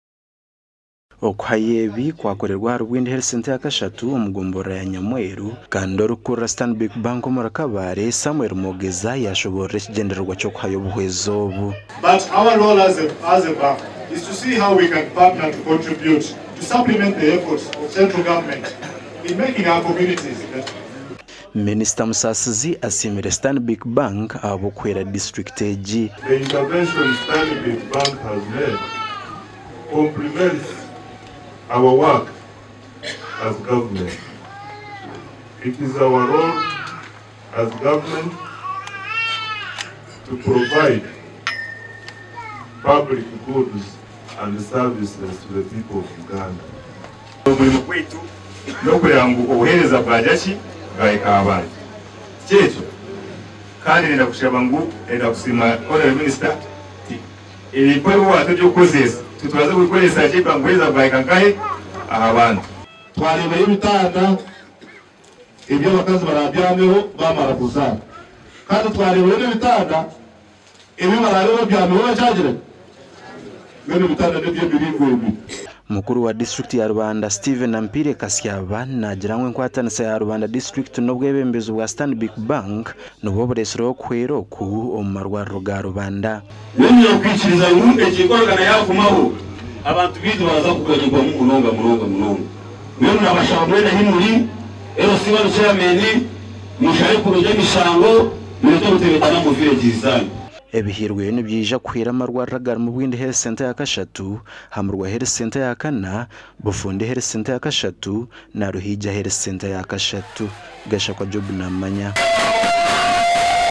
Agacencwire: Stanbic Bank ehaireyo shiringi 40,000,000 omumarwariro ga Rubanda
RUBANDA

Stanbic Bank ehaireyo shiringi 40,000,000 omumarwariro ga Rubanda